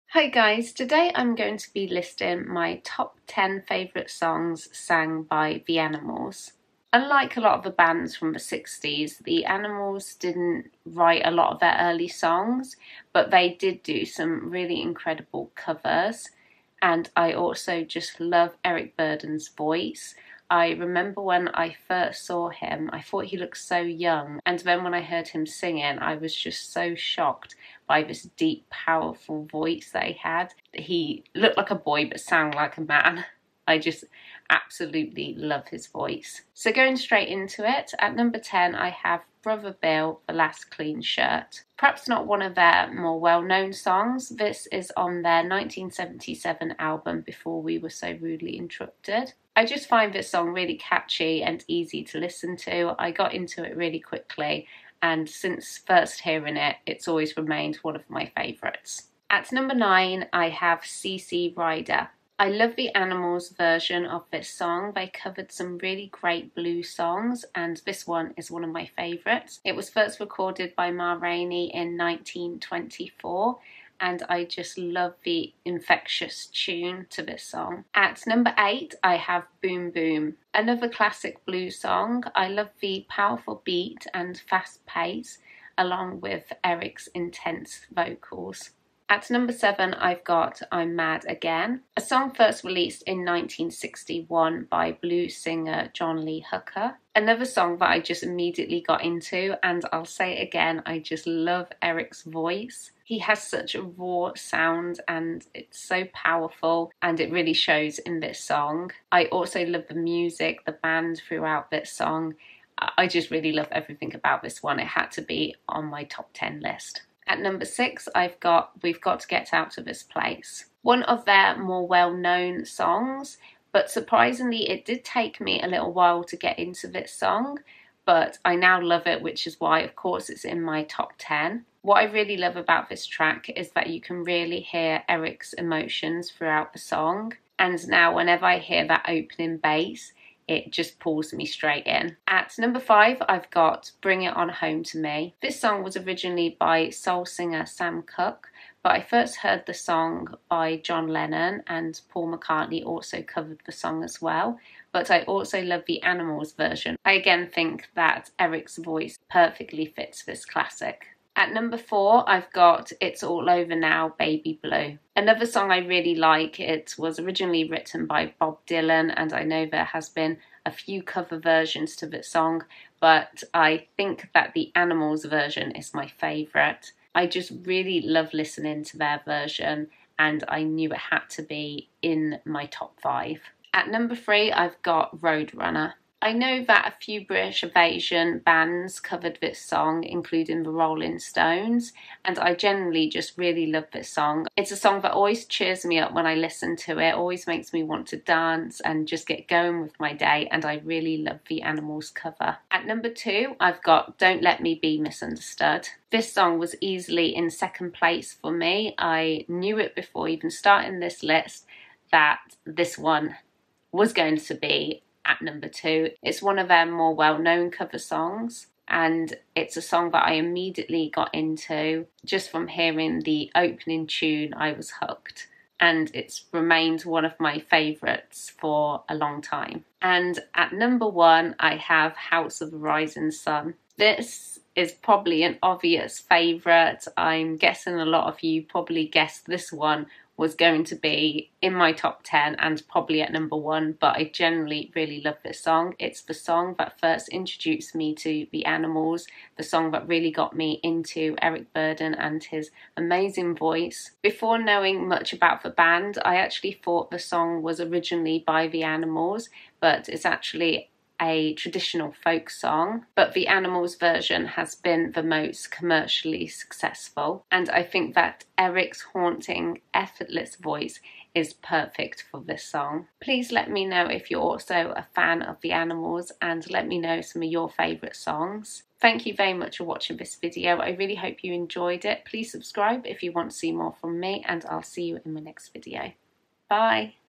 My Top 10 THE ANIMALS Songs
I list my top 10 The Animals songs.

Please check out my novel, Beegan Burch, available to download from Amazon: